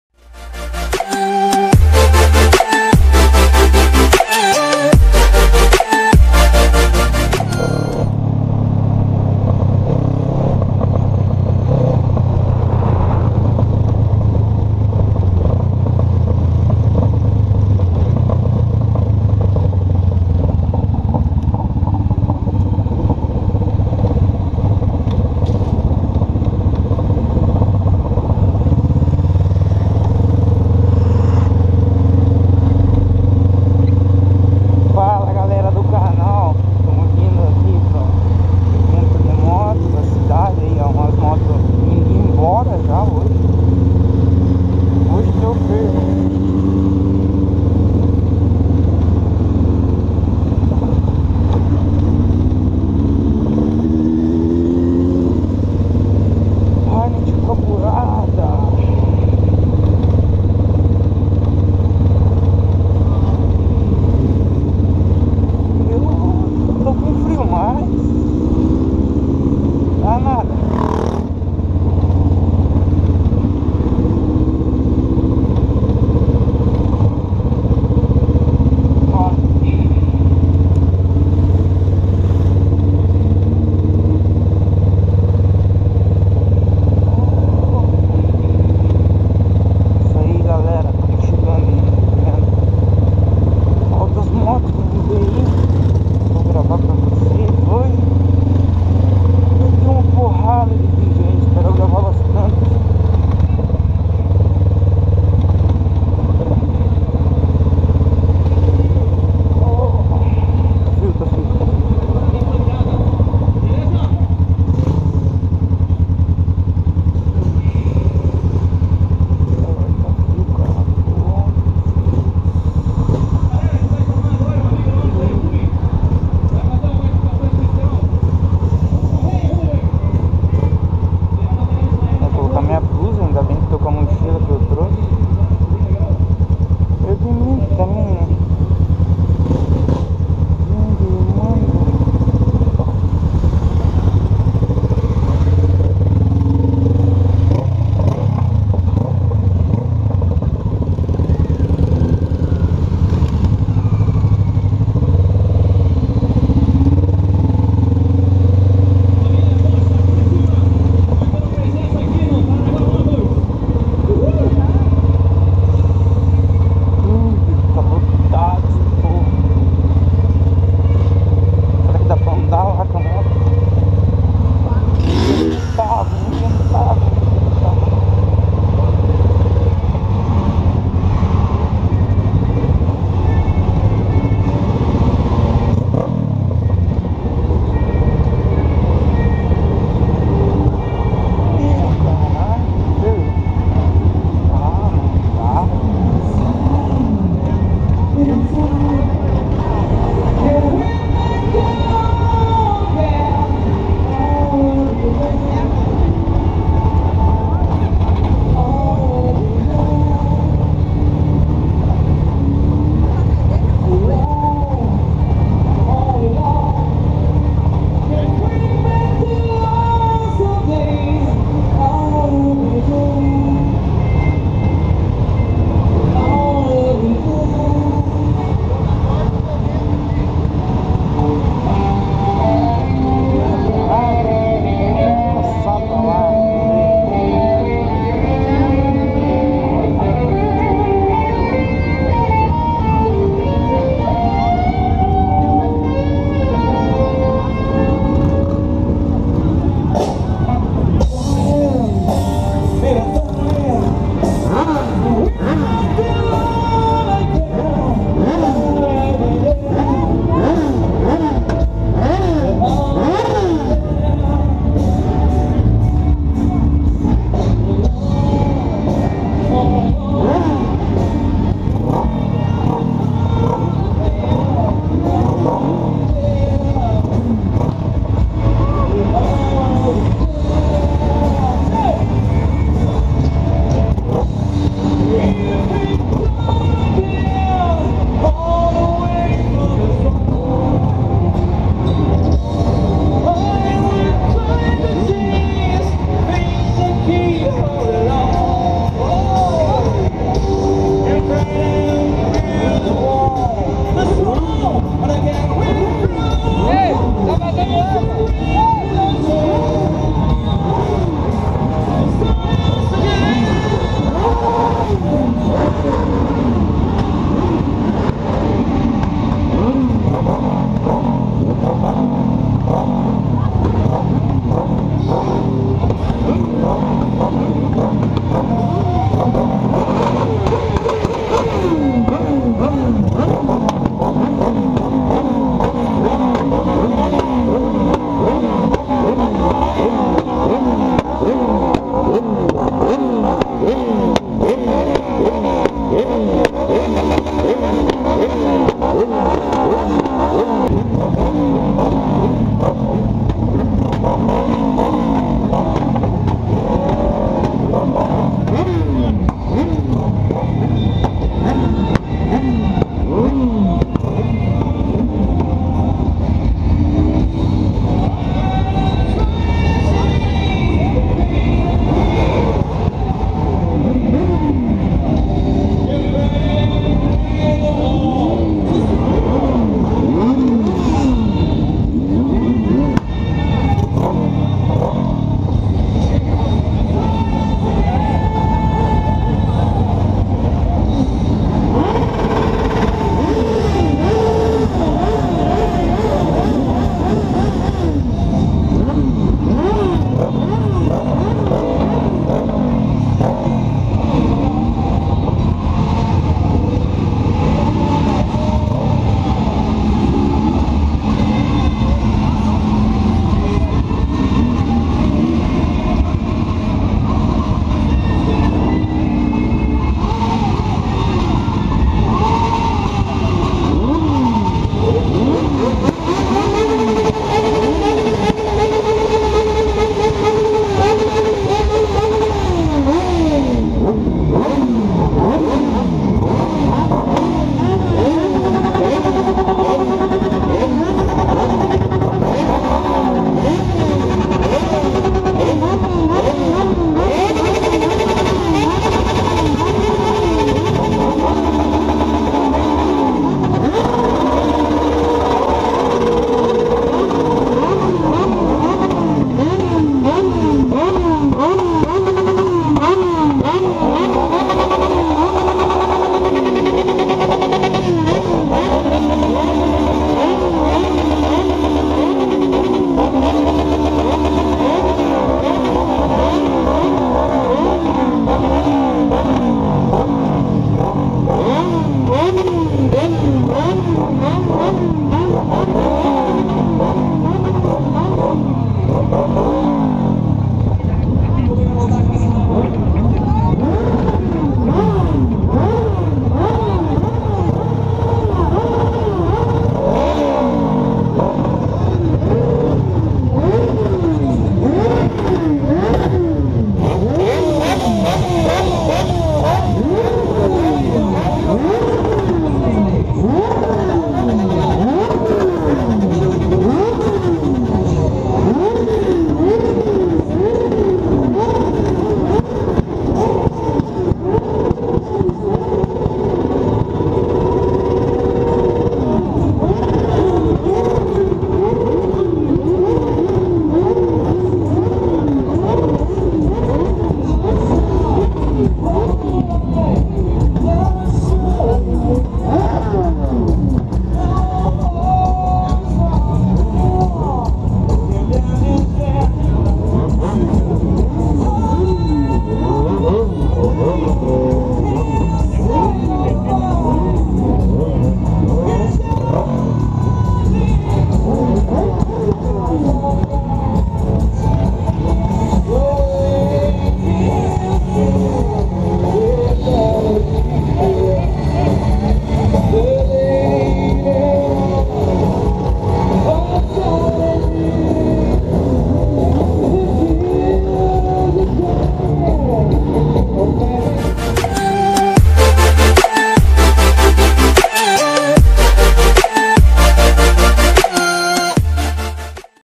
PARANAGUÁ MOTOS (PARTE 1)
Paranaguá motos 2019, o evento estava top demais, fiquem ligados que vai ter mais videos para ser postados do evento!

Motos do Canal: CB500F - Vermelha e preta
                              NinjaR - Verde e preta

Na filmagem: Luan Zela
Edição:  Luan Zela

PARANAGUÁ - PARANÁ
BRASIL